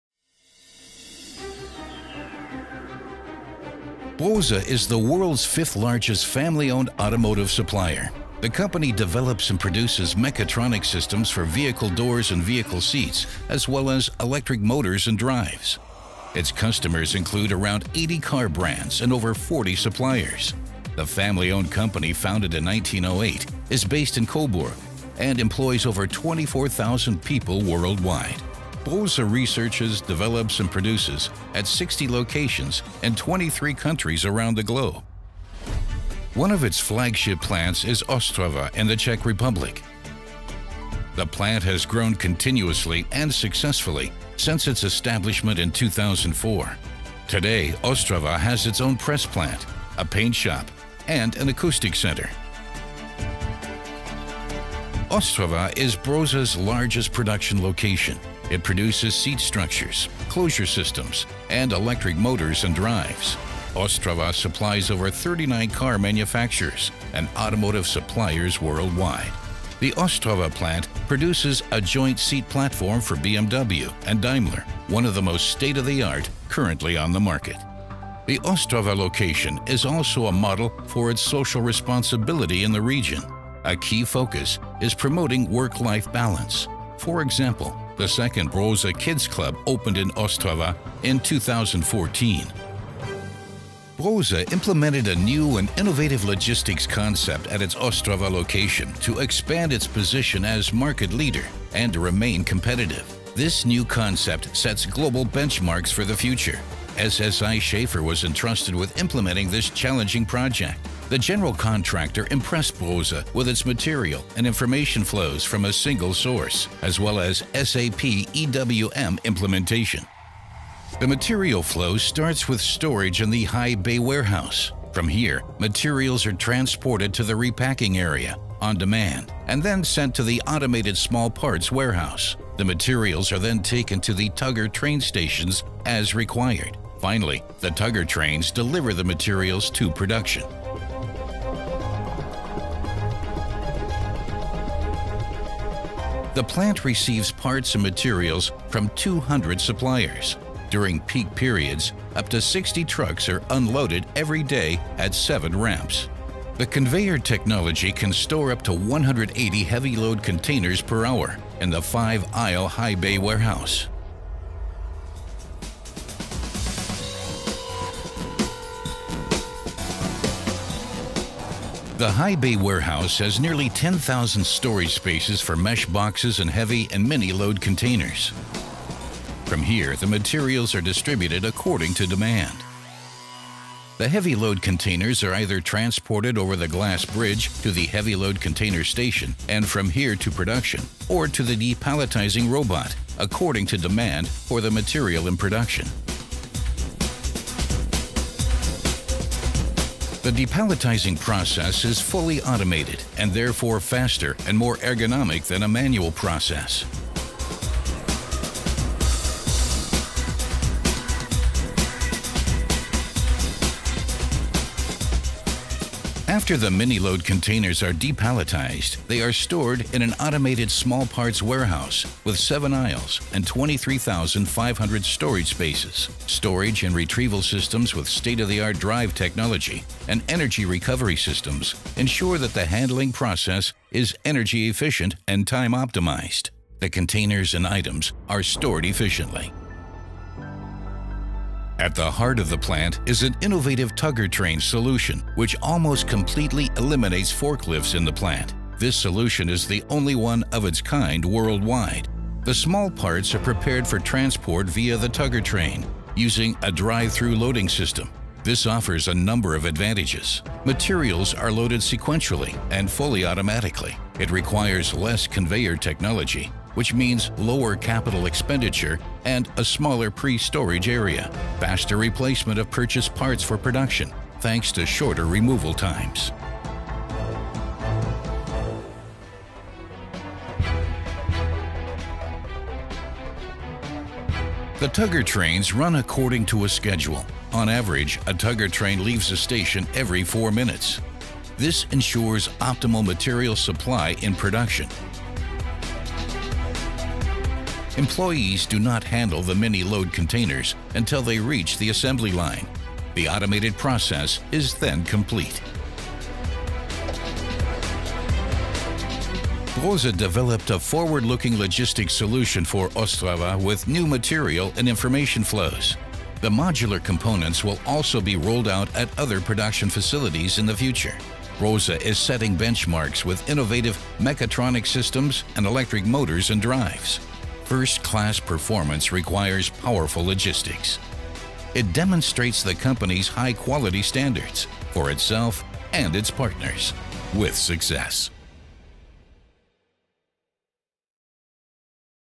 Brose Ostrava, Czech Republic – first class performance requires powerful logistics
Ostrava is Brose’s largest production location. It produces seat structures, closure systems and electric motors and drives. Ostrava supplies over 30 car manufactures and automotive suppliers worldwide. Brose implemented a new and innovative logistics concept at its Ostrava location to expand its position as market leader and to remain competitive. This new concept sets global benchmarks for the future.